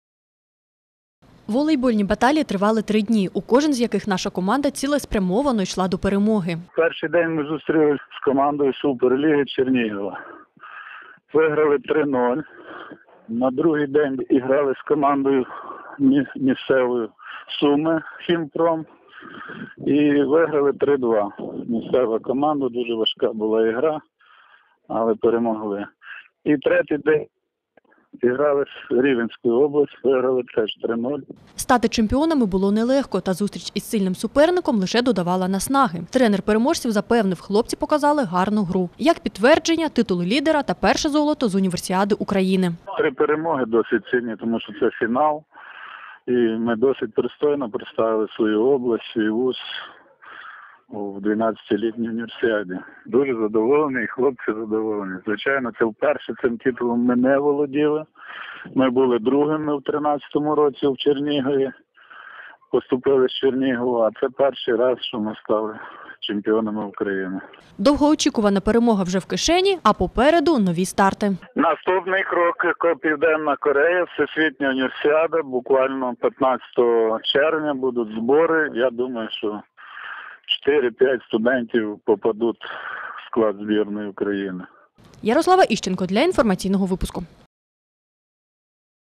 ХОДТРК "Поділля-центр" - Волейбол
Волейболісти Хмельниччини повернулися з перемогою з 12 літньої Універсіади, що днями закінчилася в Сумах. Золото краю принесли вихованці Кам’янець-Подільського національного університету імені Івана Огієнка. На майданчику подоляни обіграли досвідчених суперників із Сумської, Рівненської та Чернігівської областей.